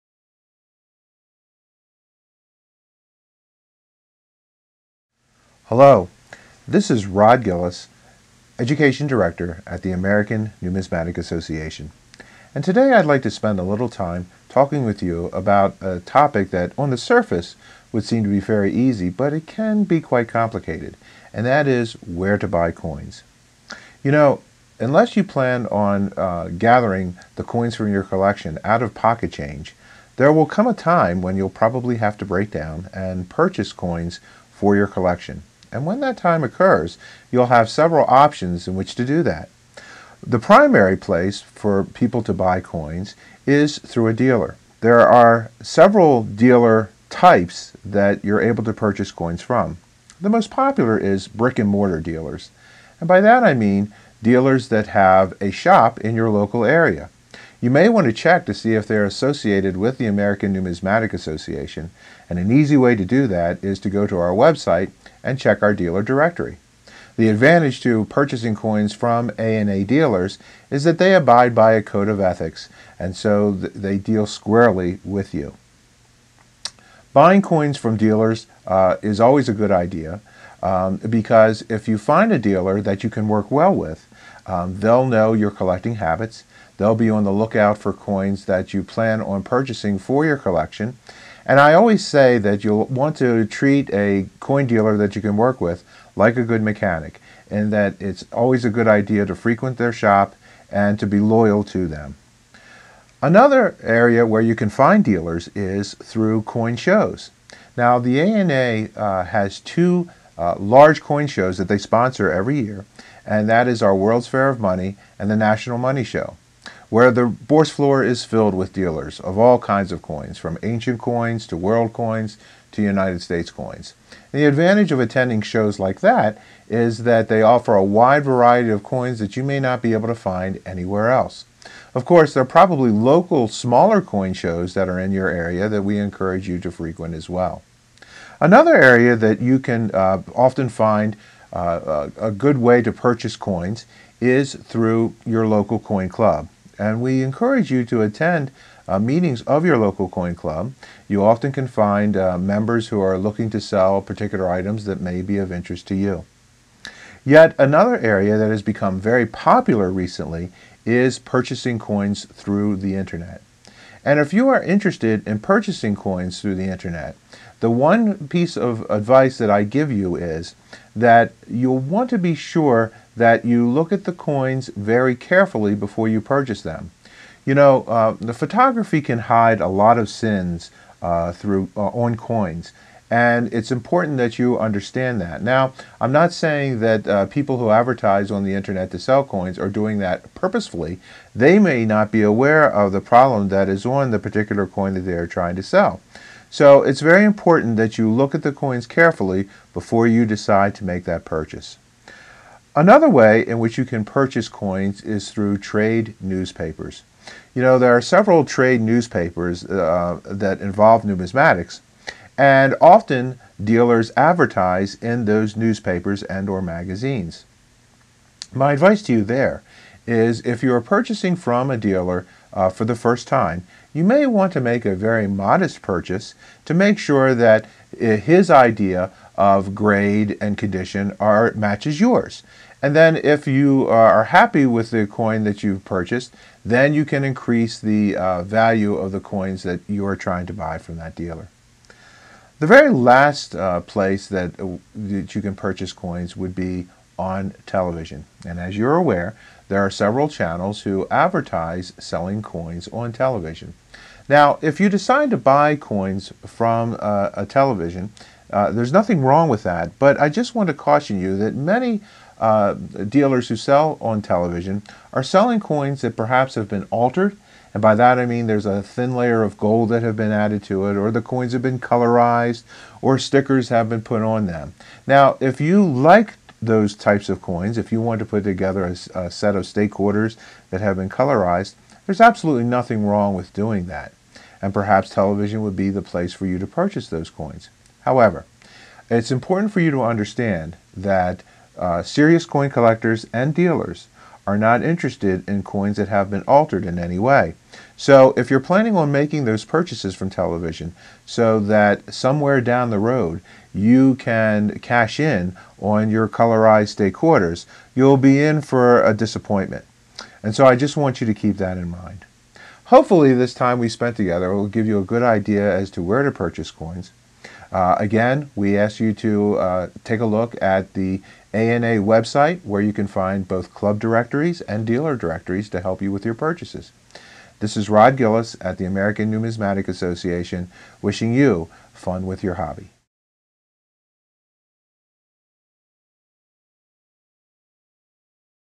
Where to Buy Collectible Coins | Beginner & Expert Tips
Trusted places to buy coins for collecting: Beginner & Expert Tips from the ANA!

Looking to enhance your coin collection? Join Rod Gillis, former Education Director at the American Numismatic Association, as he shares expert insights on where to buy collectible coins and trusted places to buy coins. In this comprehensive video, Rod explores the best and most reliable sources for collectors, from reputable local dealers and coin shows to online platforms and coin clubs. Learn how to avoid common pitfalls and ensure you're acquiring authentic coins that align with your collecting goals. Watch now to make informed purchases and elevate your numismatic journey!

00:00 Introduction
00:39 Dealers
01:42 Coin Shows
02:17 Coin Clubs
02:37 Online
03:22 Trade Newspapers
04:02 Television